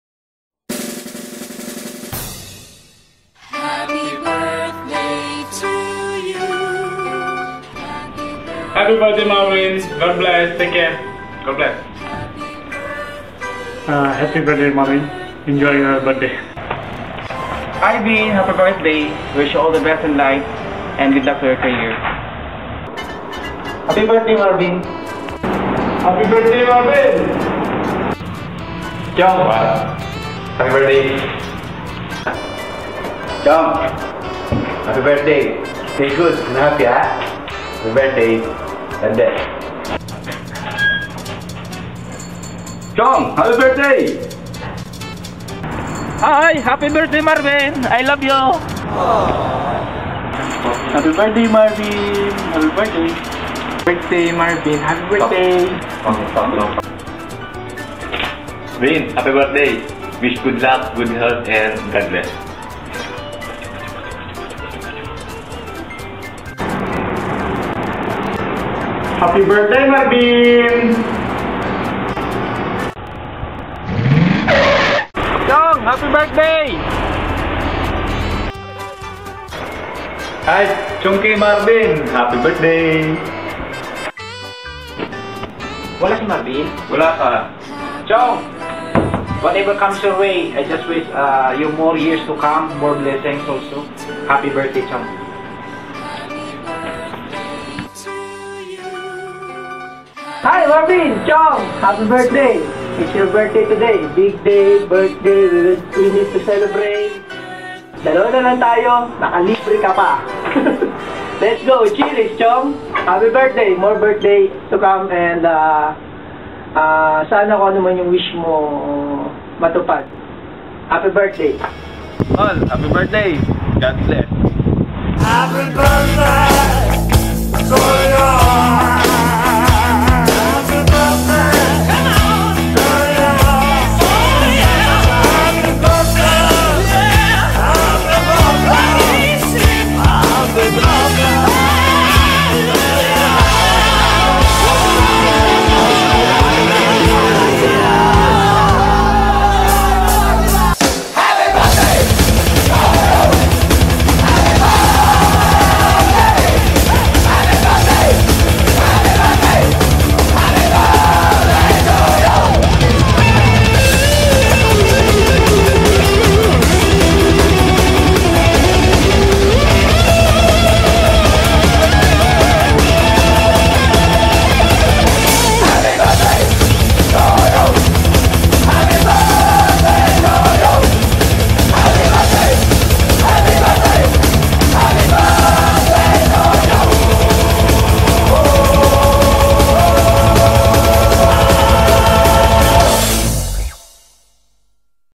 Happy Birthday Marvin
This video is for Marvin Robinson on his birthday.  Happy birthday, Marvin.  Wish you good luck good health and God bless.